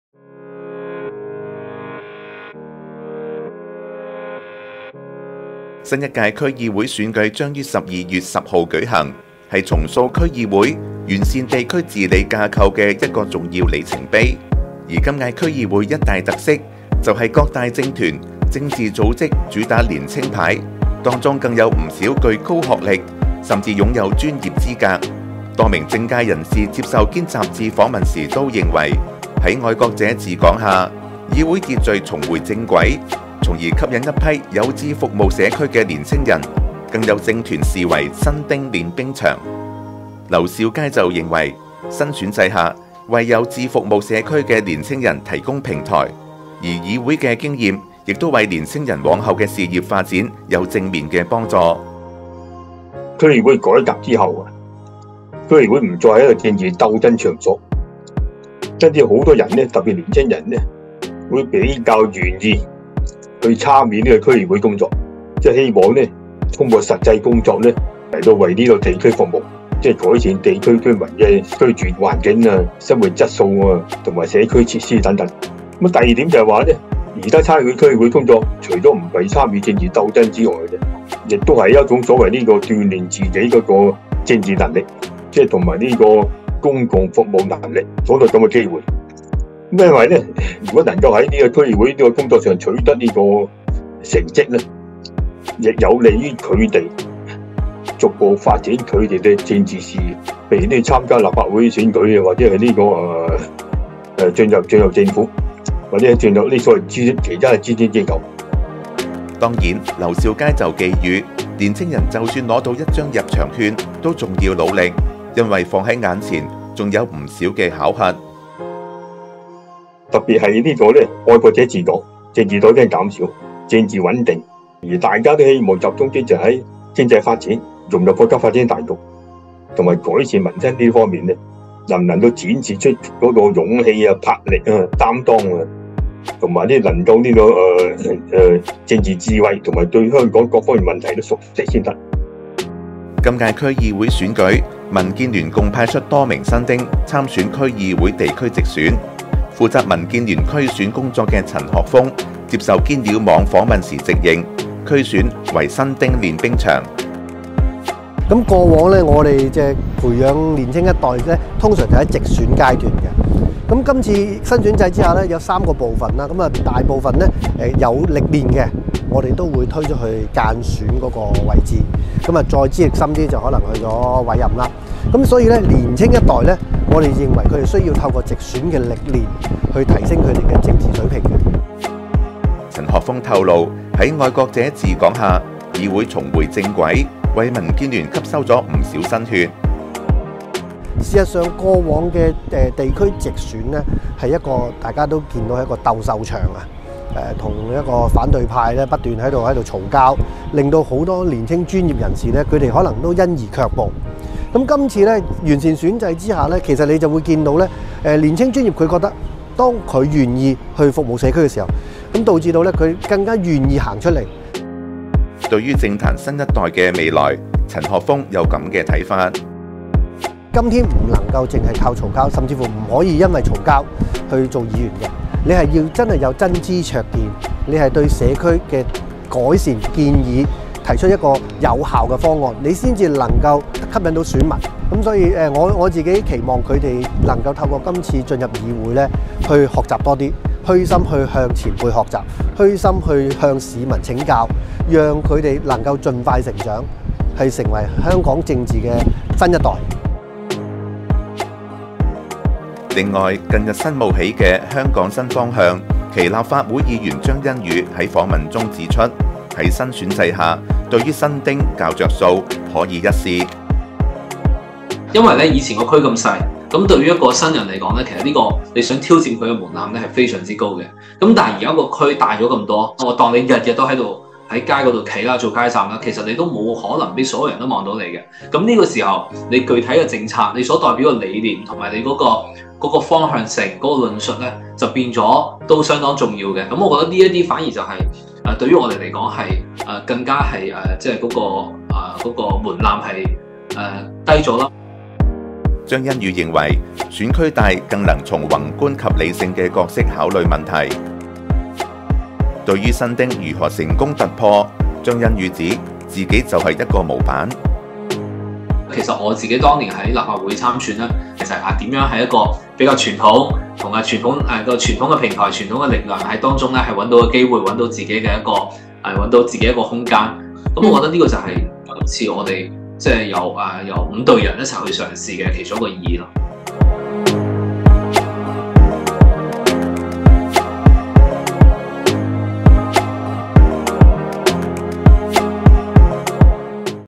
【區會新貌】新一屆區議會新亮點 候選人年輕化高學歷 政黨練兵培育人才
新一屆區議會選舉，將於12月10日舉行，也是重塑區議會完善地區治理架構的一個重要里程碑。今屆區議會選舉有一大特色，就是各大政團、政治組織主打年輕牌，當中不少更具高學歷，甚至擁有專業資格。多名政界人士分析，主要原因在愛國者治港的原則下，議會秩序重回正軌，政治暴力絕跡，從而吸引一批有意服務社區的年輕人躍躍欲試。
其中，在44個選區均派員參選的民建聯直言，視區議會直選為人才培養的主戰場，冀透過直選的洗禮，增加本身的政治實力。而新冒起政團「香港新方向」就派出五名「90後」和「95後」的新人參選，並主打高學歷及專業背景，冀借助新的選舉模式，為政治「新丁」打開一條出路。
全國港澳研究會顧問劉兆佳就認為，有新血加入，當然對香港政制發展有一定的幫助，但特區政府不能視為政治人才庫，必須繼續重點培養人才。